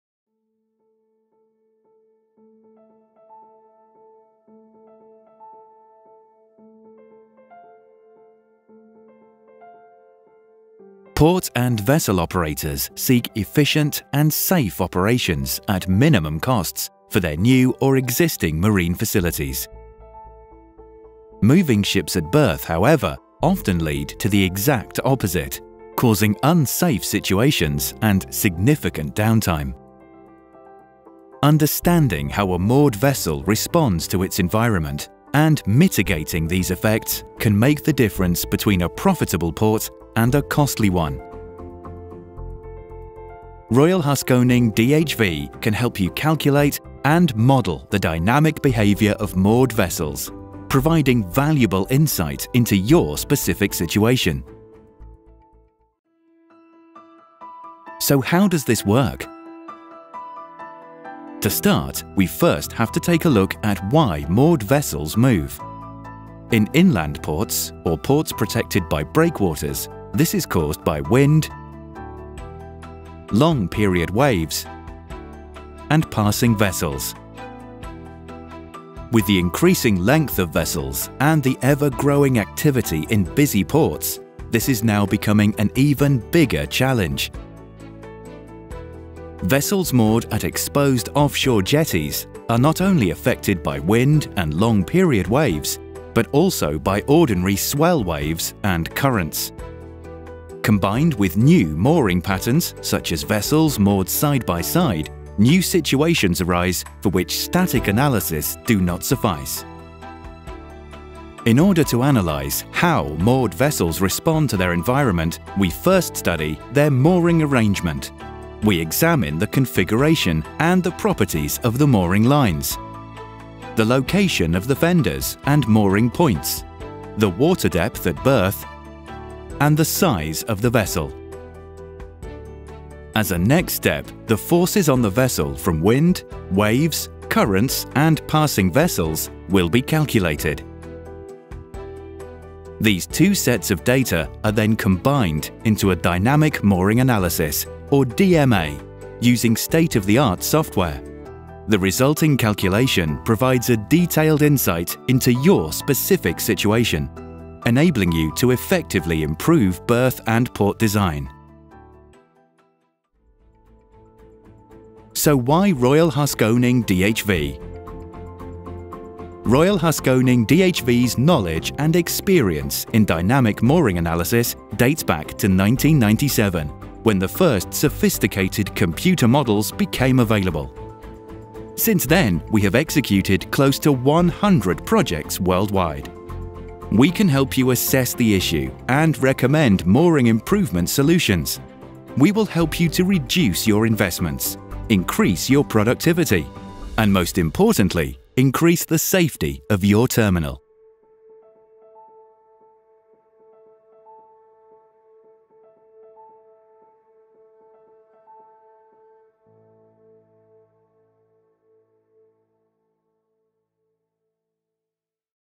Dynamic Mooring Analysis (DMA) | Royal HaskoningDHV
Calculating how a moored vessel responds to its environment has become an essential part of effective harbour management. Mitigating the effects of passing ships, high wind, wave and current conditions on moored vessels can make the difference between a profitable port and a costly one. So when it comes to deciding how to moor deep-draft vessels, a little help from the experts may make all the difference.